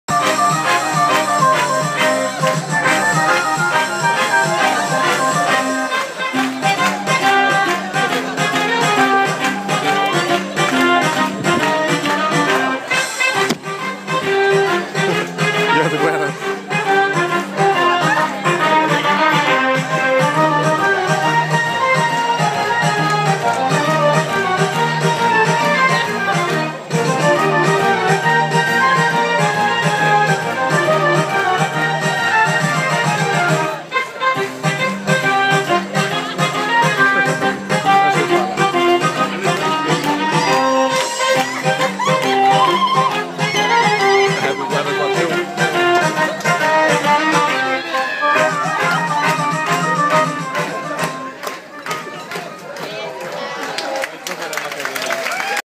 Dinner on Royal Caribbean
Fun night in the restaurant. The restaurant staffs gave us an entertainment.